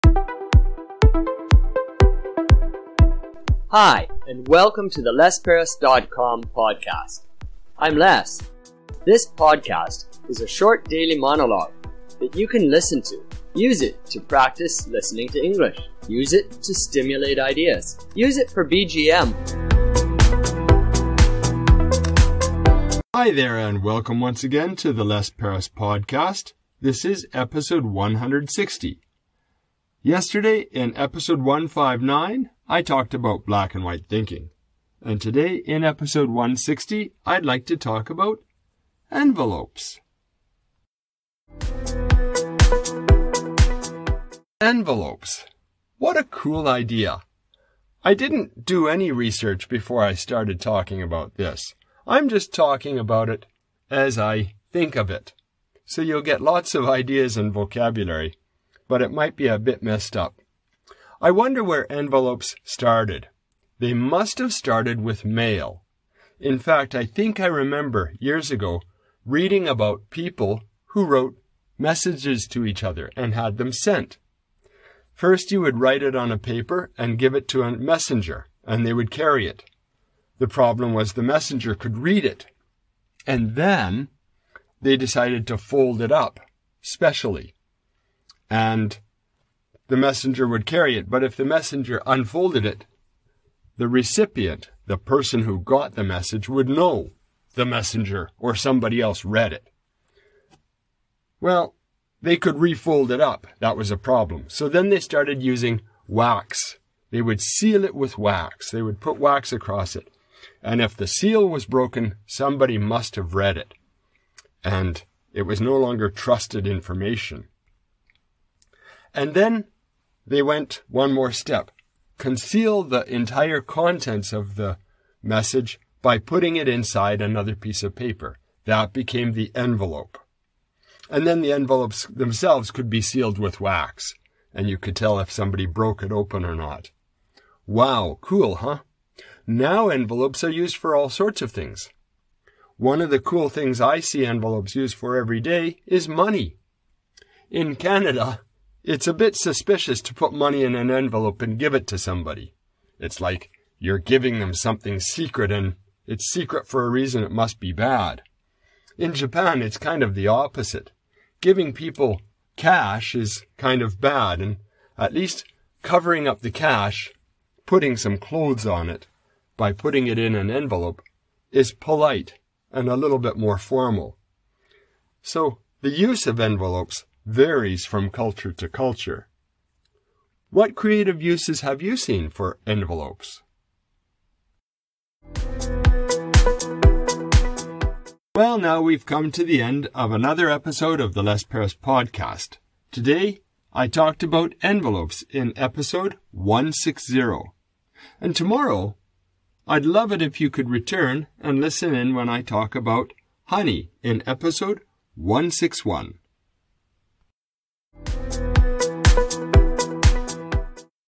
Envelopes
Envelopes. What a cool idea. I didn't do any research before I started talking about this. I'm just talking about it as I think of it. So you'll get lots of ideas and vocabulary. But it might be a bit messed up. I wonder where envelopes started. They must have started with mail. In fact, I think, I remember years ago reading about people who wrote messages to each other and had them sent first, you would write it on a paper and give it to a messenger and they would carry it. The problem was the messenger could read it and then they decided to fold it up special and the messenger would carry it. But if the messenger unfolded it, the recipient, the person who got the message would know the messenger or somebody else read it. Well, they could refold it up. That was a problem. So then they started using wax, they would seal it with wax, they would put wax across it. And if the seal was broken, somebody must have read it and it was no longer trusted information. And then they went one more step, conceal the entire contents of the message by putting it inside another piece of paper that became the envelope and then the envelopes themselves could be sealed with wax and you could tell if somebody broke it open or not. Wow. Cool. Huh. Now, envelopes are used for all sorts of things. One of the cool things I see envelopes used for every day is money in Canada. It's a bit suspicious to put money in an envelope and give it to somebody. It's like you're giving them something secret and it's secret for a reason. It must be bad in Japan. It's kind of the opposite. Giving people cash is kind of bad and at least covering up the cash, putting some clothes on it by putting it in an envelope is polite and a little bit more formal. So the use of envelopes varies from culture to culture. What creative uses have you seen for envelopes?